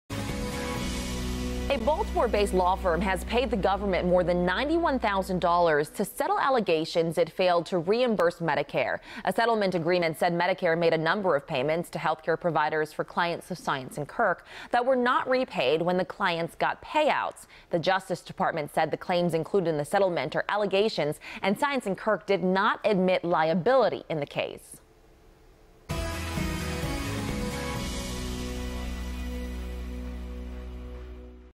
Baltimore-Based Law Firm Pays $91K Over Allegations It Failed To Reimburse Medicare
A settlement agreement said Medicare made a number of conditional payments to healthcare providers for clients of Saiontz & Kirk that the government said were not repaid when the clients got payouts from settlements or judgments in their favor.